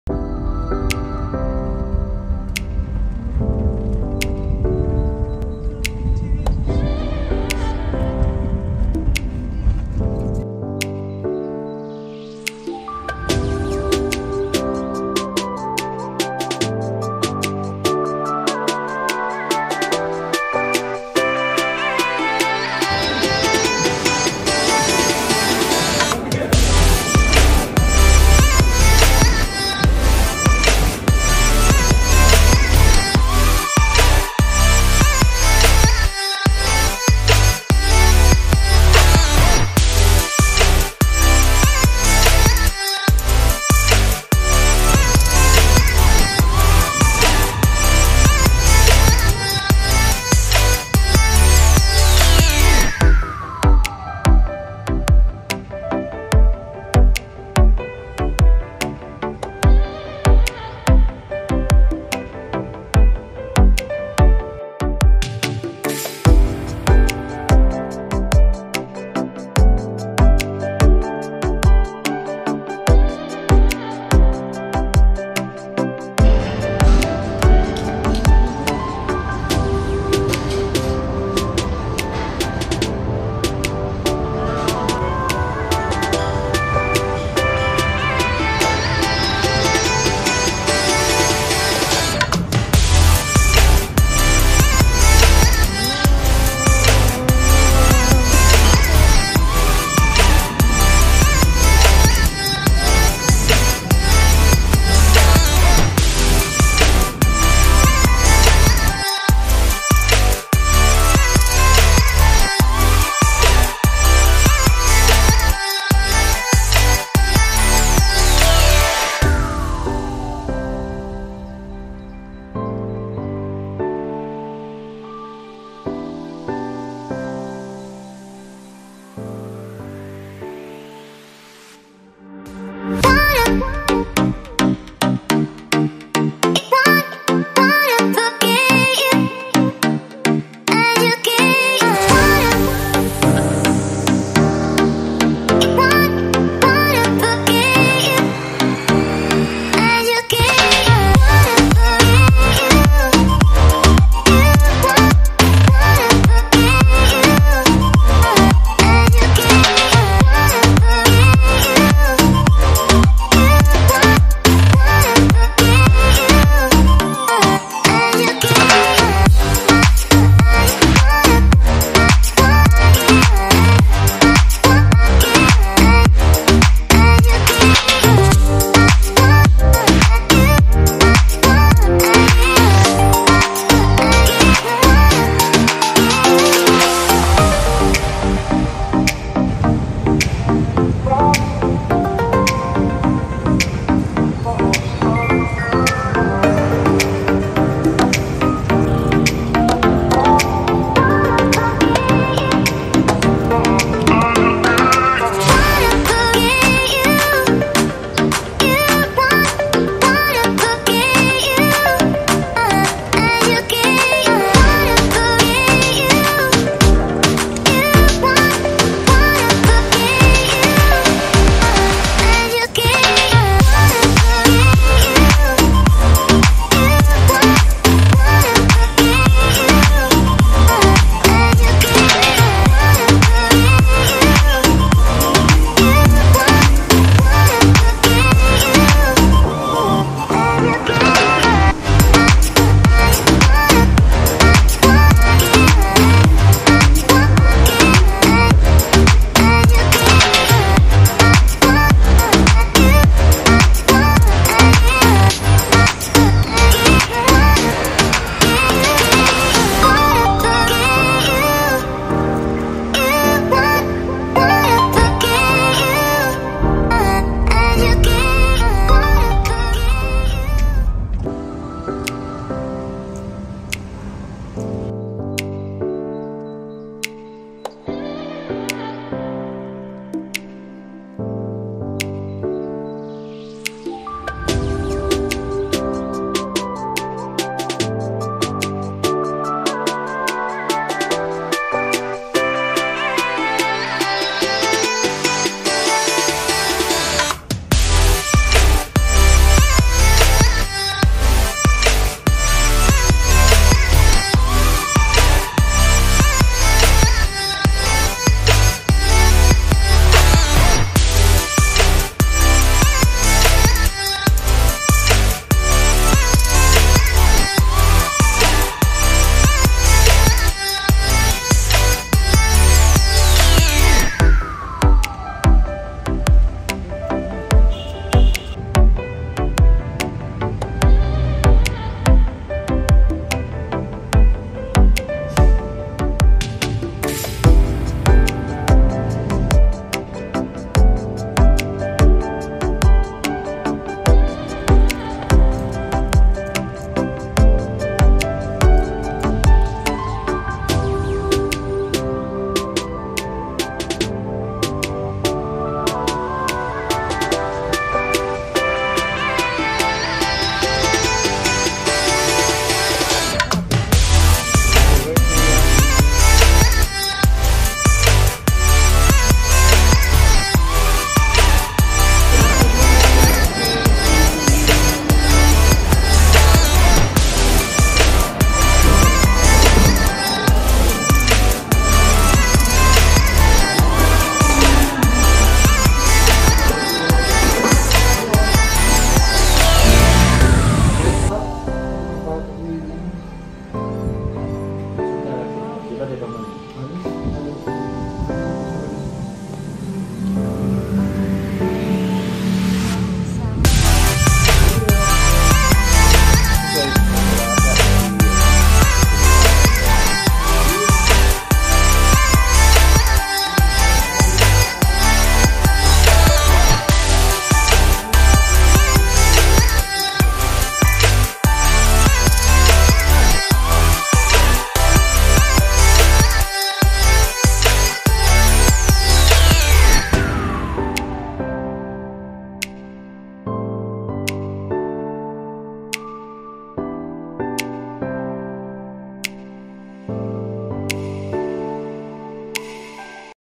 Happy Birthday My Yani & Hubb | Uptown Mall BGC |Jenna Sebbey
Happiest Birthday to my favorite person on this planet, my husband ❤️ I love you forever and ever xx

Musician: @iksonmusic

Musician: @iksonmusic